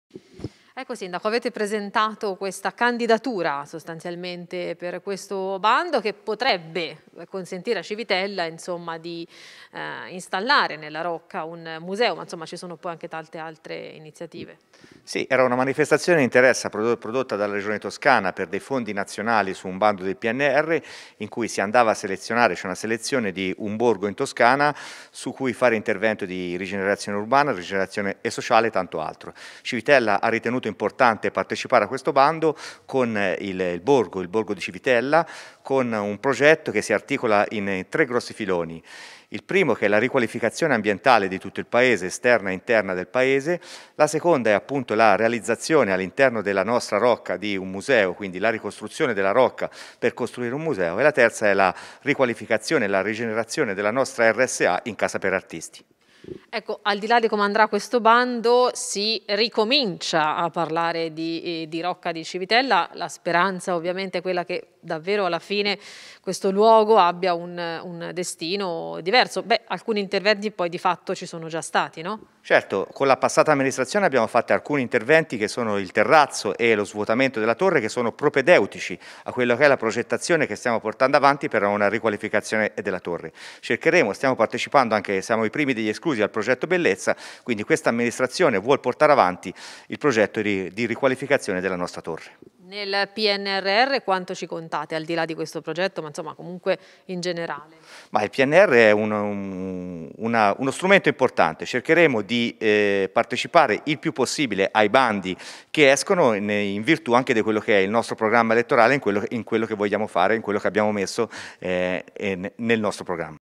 Civitella si candida al bando per la rigenerazione urbana
Civitella in Valdichiana si è candidata al concorso attraverso il quale la Regione Toscana devolverà 20 milioni di euro per il recupero del patrimonio architettonico, culturale e ambientale,  tra gli obiettivi ad esempio la trasformazione della Rocca in un museo.